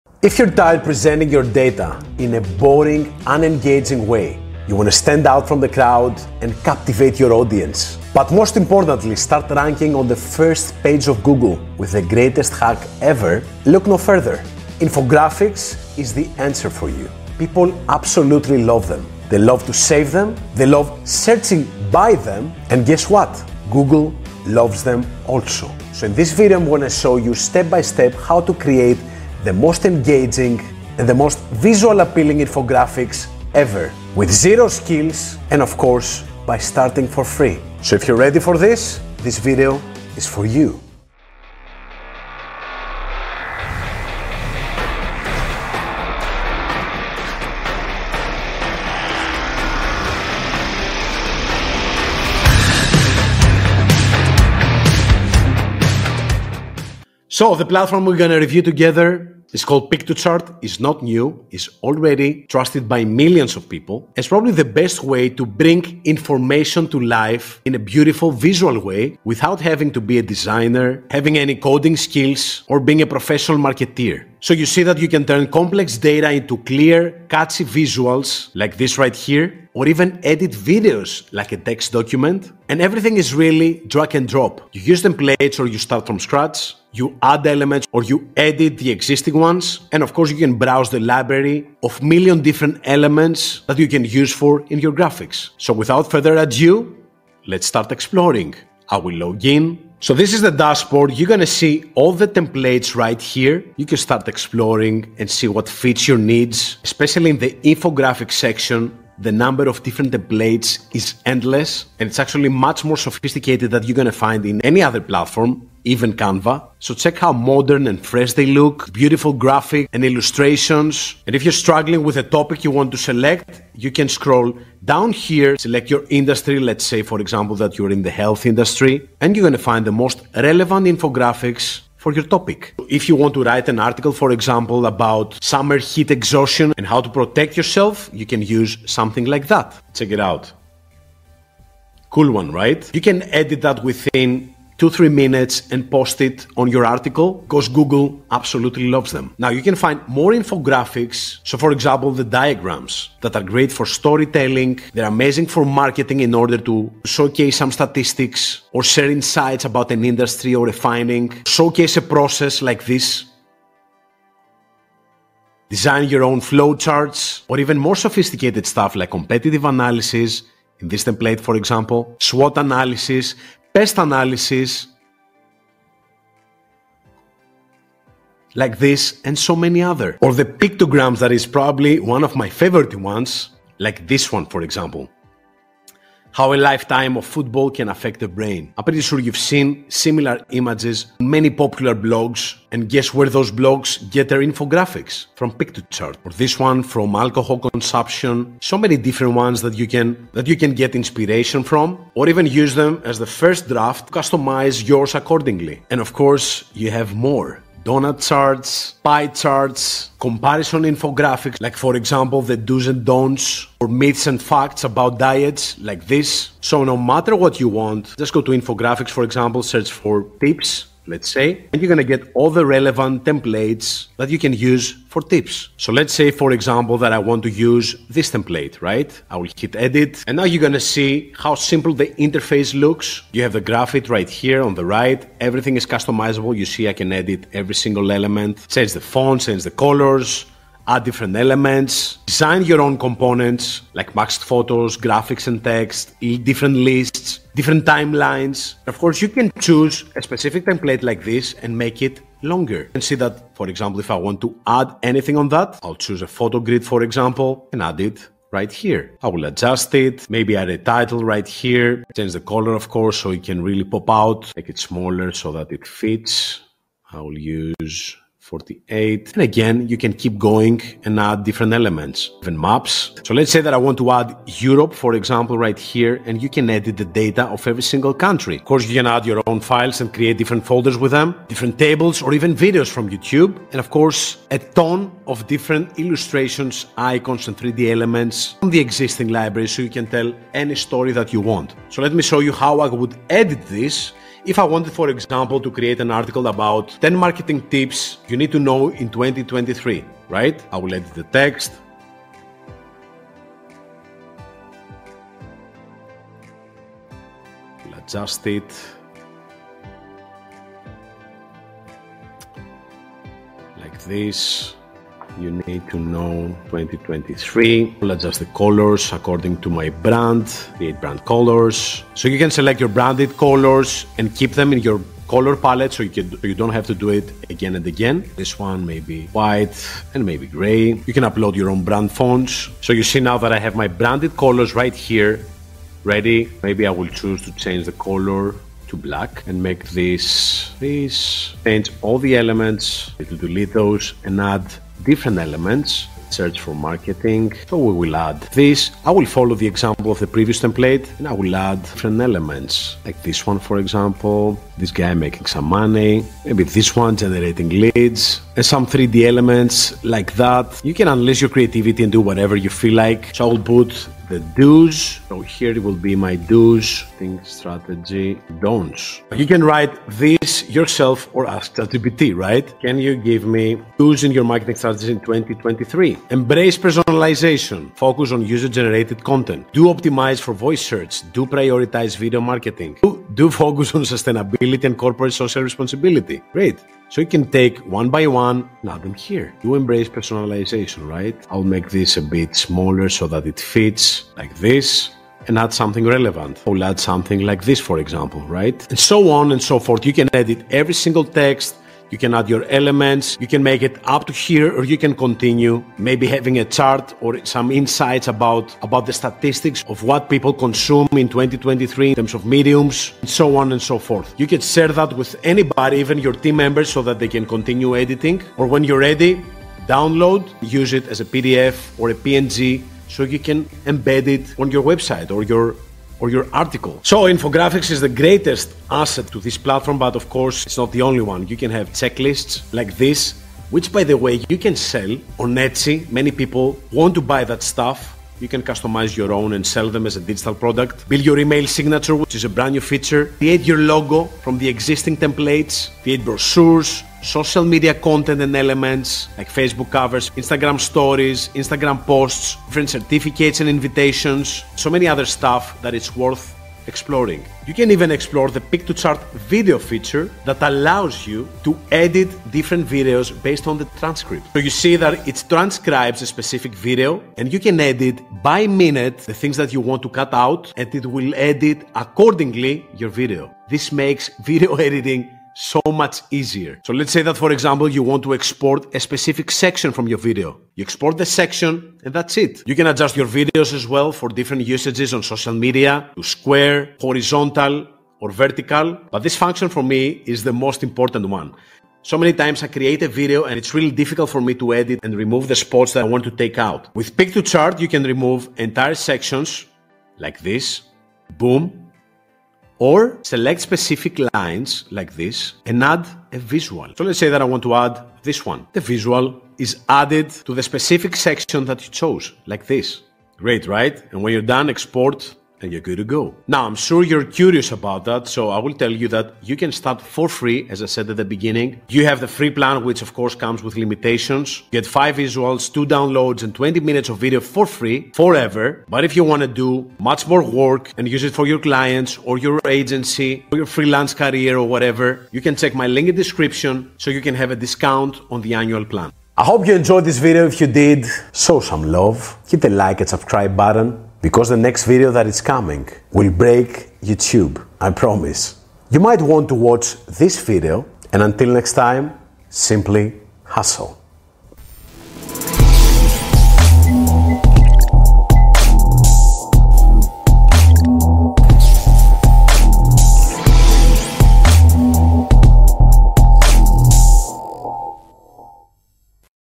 Best way to make infographics for FREE in 2023 (Piktochart full tutorial)
This is really the most sophisticated and simple way to create top-notch infographics for anything you want and one of the best ways to rank on the 1st page of Google!

- Get my Video Course to explode with short video: "2 million & 6 figures in 8 months"